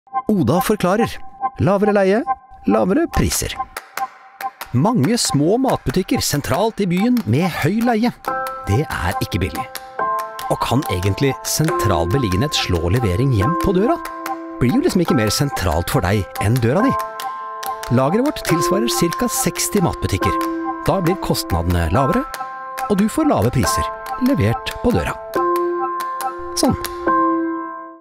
Lavere leie, lavere priser!
Mange små matbutikker sentralt i byen med høy leie, det er ikke billig. Og kan sentral beliggenhet egentlig slå levering hjem på døra? Blir jo liksom ikke mer sentralt for deg, enn døra di. Lageret vårt tilsvarer ca. 60 matbutikker. Da blir kostnadene lavere. Sånn!